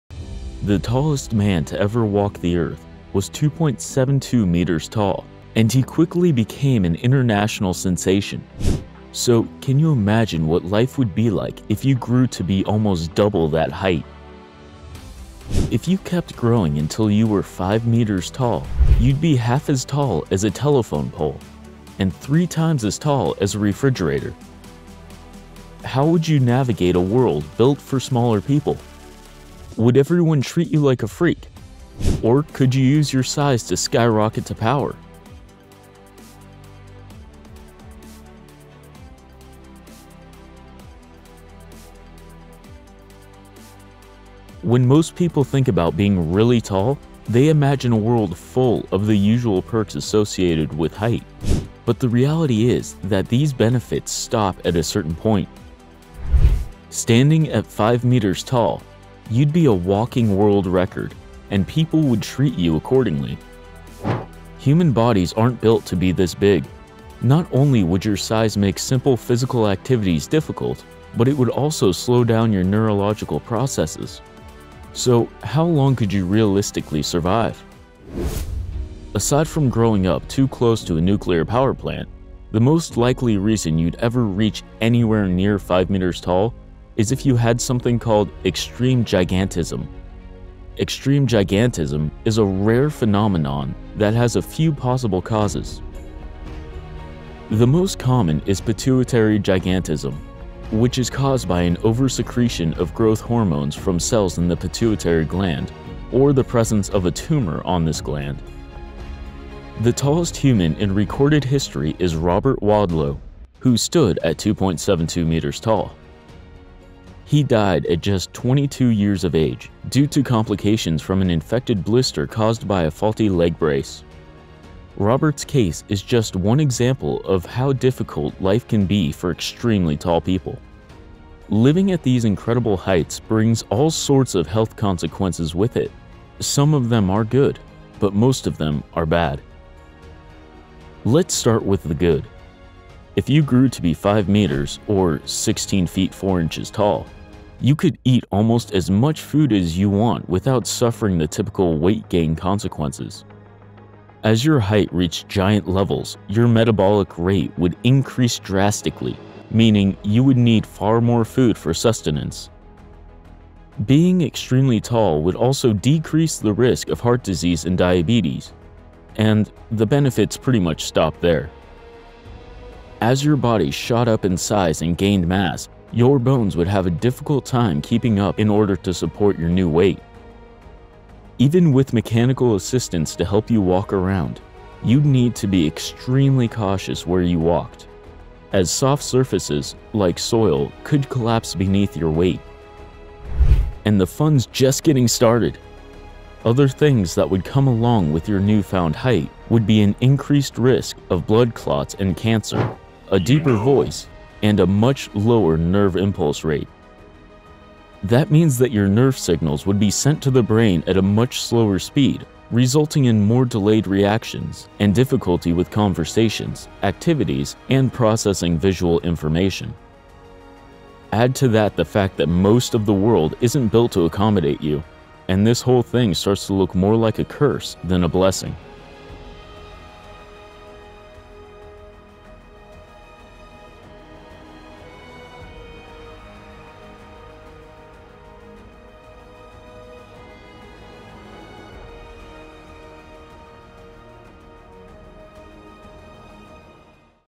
What If Humans Were 5 Meters Tall?
The tallest man to ever walk the Earth was 2.72 m  tall, and he quickly became an international sensation.
So can you imagine what life would be like if you grew to be almost double that height?
If you kept growing until you were 5 m tall, you’d be half as tall as a telephone pole,and three times as tall as a refrigerator,. How would you navigate a world built for smaller people?
Would everyone treat you like a freak? Or could you use your size to skyrocket to power?
When most people think of being really tall, they imagine a world full of the usual perks associated with height, but the reality is that these benefits stop at a certain point. Standing at 5 tall, you’d be a walking world record, and people would treat you accordingly.
Human bodies aren’t built to be this big; not only would your size make simple physical activities difficult, but it would also slow down your neurological processes. So how long could you realistically survive?